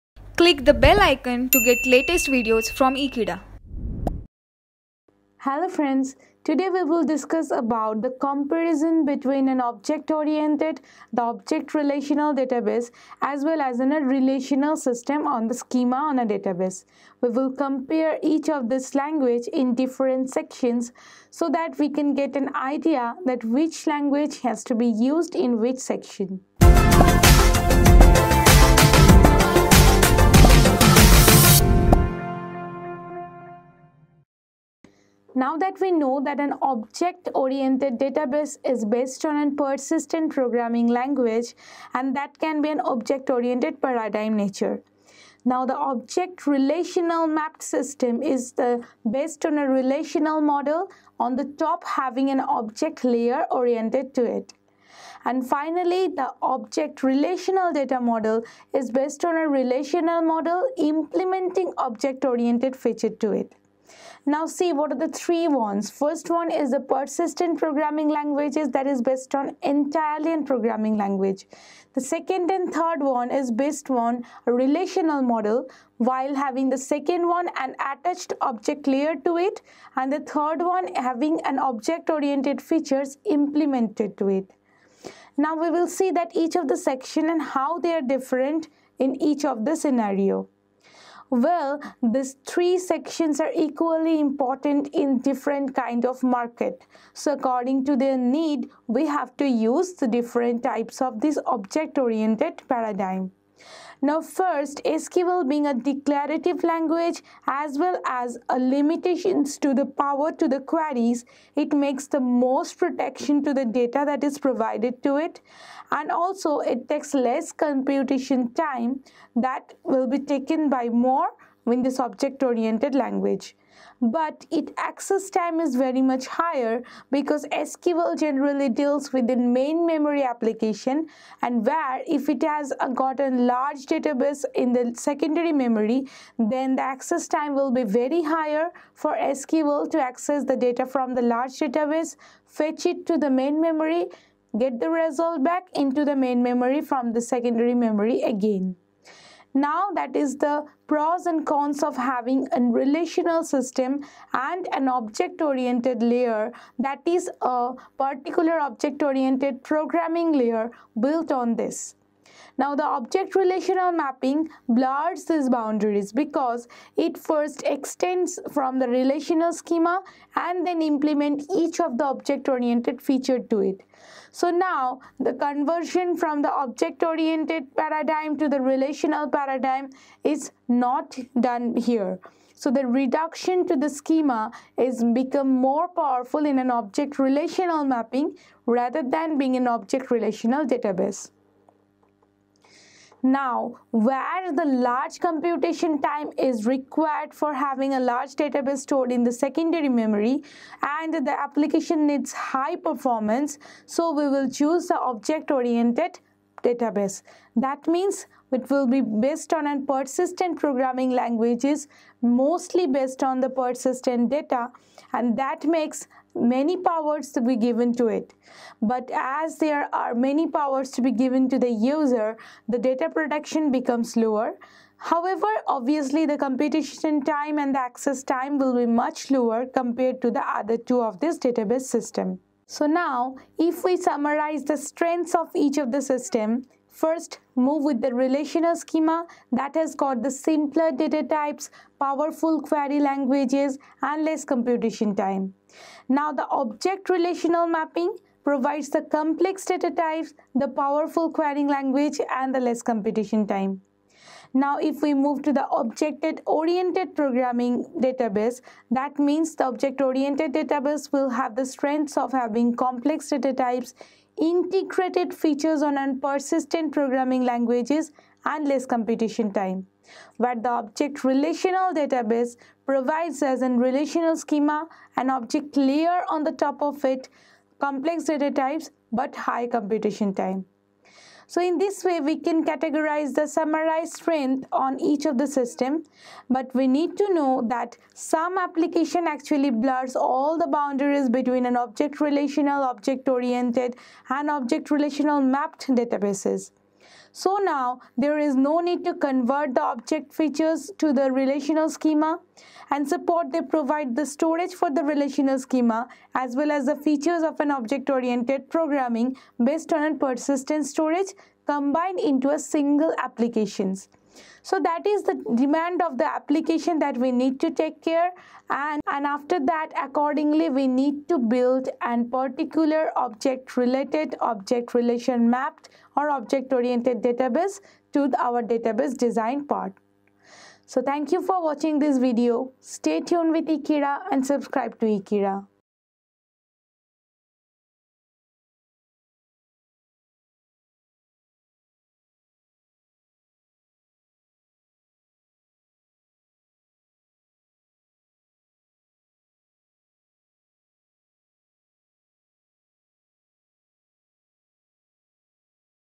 Object Oriented VS Object Relational Database - Object Based Databases - Database Management System
Subject - Database Management System

Video Name -

Chapter - Object Based Databases

Faculty - Prof. Sangeeta Dey

Happy Learning